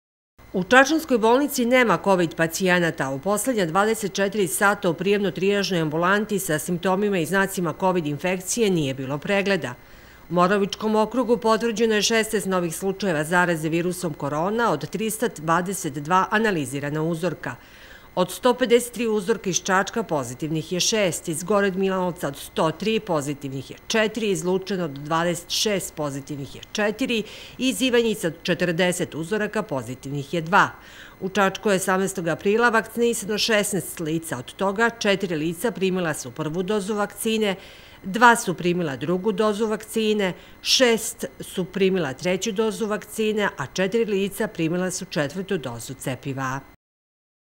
TV TELEMARK - I DALJE BEZ KOVID PACIJENATA U ČAČANSKOJ BOLNICI 19 04 2022
U čačanskoj bolnici nema kovid pacijenata, a u poslednja 24 sata u Čačku je potvrđeno šest novih slučajeva zaraze koronavirusom od 153 analizirana uzorka.